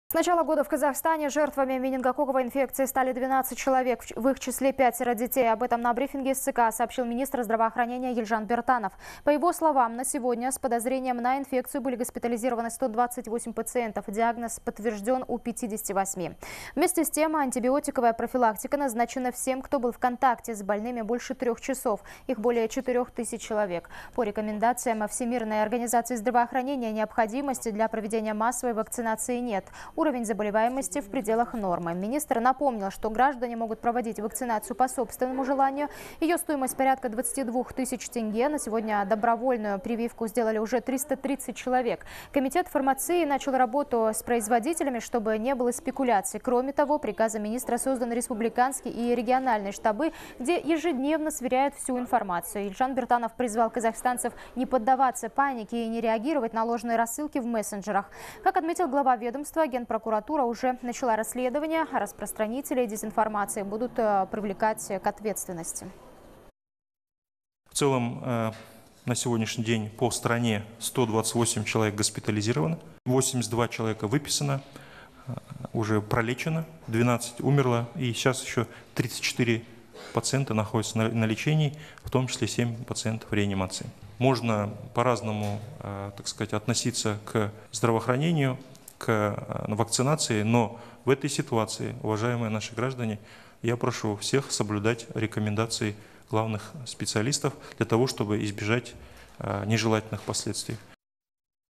Елжан Биртанов: Пик заболеваемости менингококковой инфекцией зафиксирован в Алматы
На сегодня в РК с начала года уже зафиксировано 58 случаев заболевания менингитом, 18 из них дети до 14 лет, 12 случаев с летальным исходом, 5 из них дети. Об этом сообщил министр здравоохранения РК Елжан Биртанов в ходе брифинга СЦК.